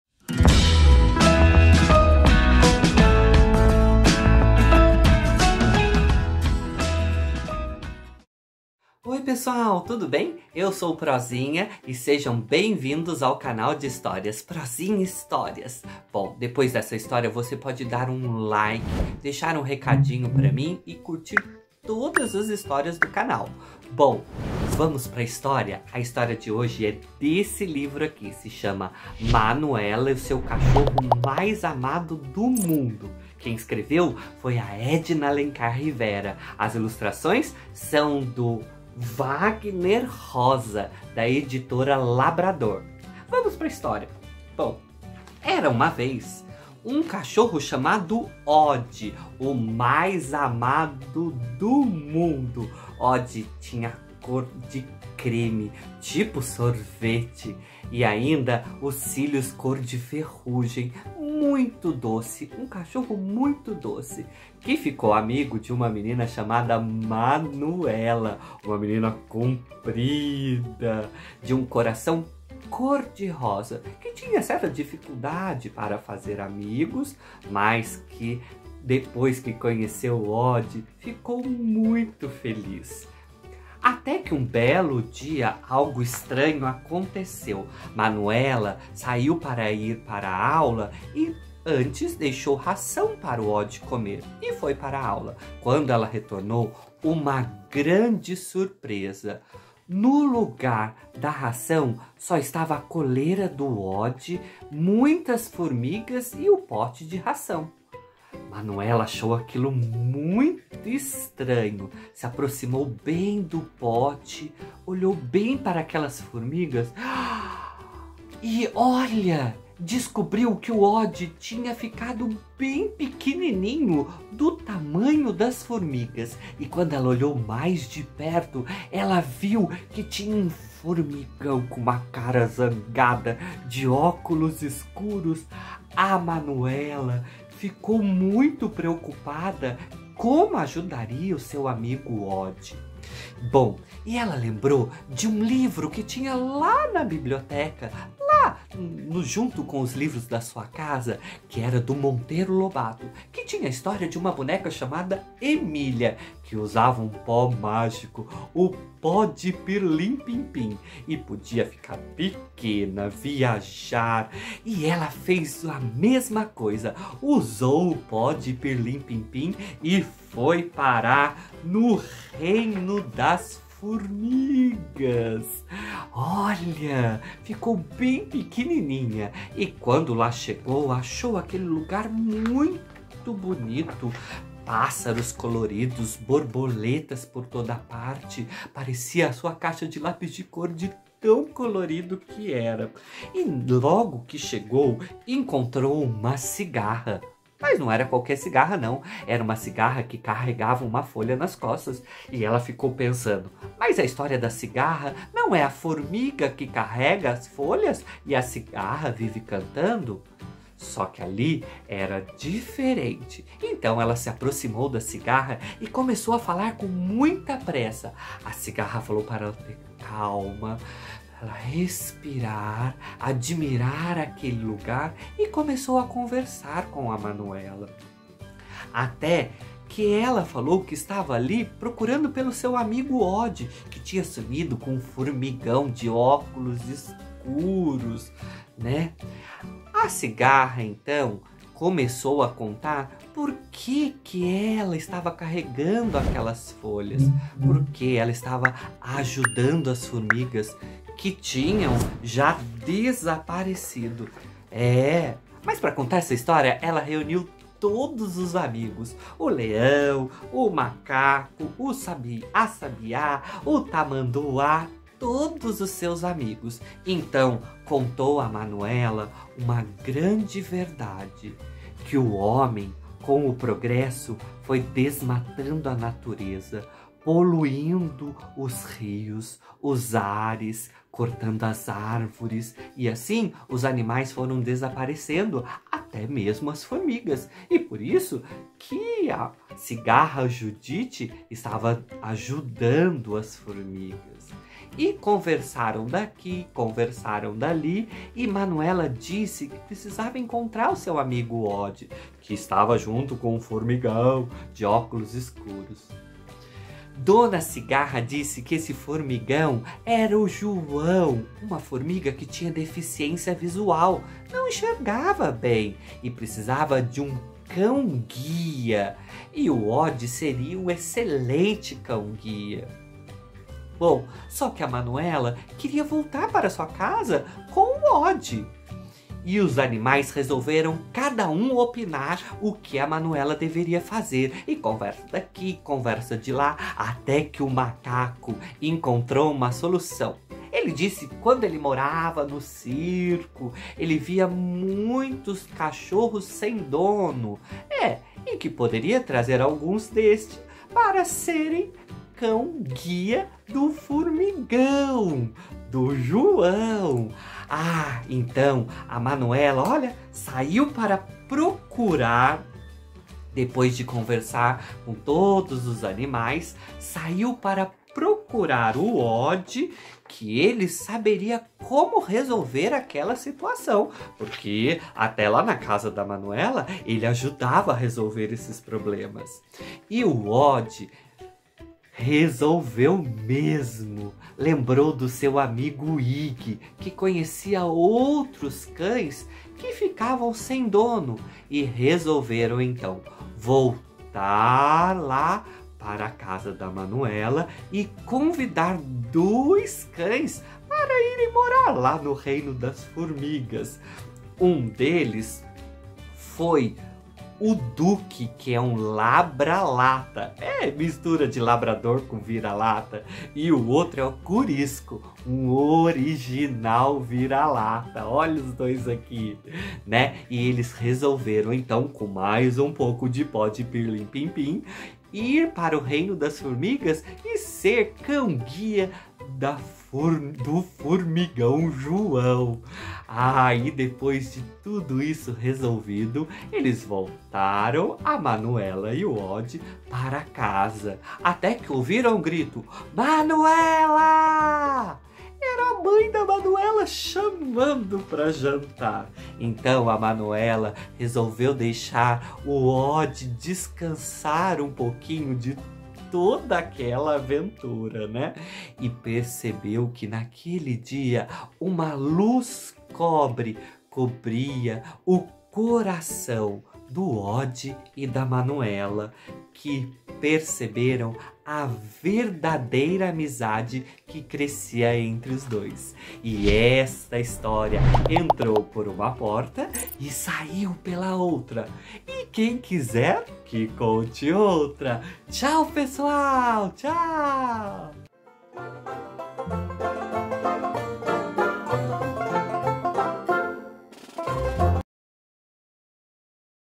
👧🏻🐶Manuela e seu cachorro mais amado do mundo📚Autora: Edna Alencar Rivera📚🖌Ilustração: Wagner Roza🖍️
EDITORA LABRADOR
📚📚Um dedinho de Prosa:
👧🏻🐶O cachorro Odie é o melhor amigo que uma menina poderia desejar: um grande companheiro de aventuras e brincadeiras!👧🏻🐶 Manuela não poderia estar mais feliz, pois Iana de todo o coração.  Mas certo dia, Odie some misteriosamente !! 🐶

🐶📚 Uma história para celebrar a amizade e as novas descobertas!!👧🏻📚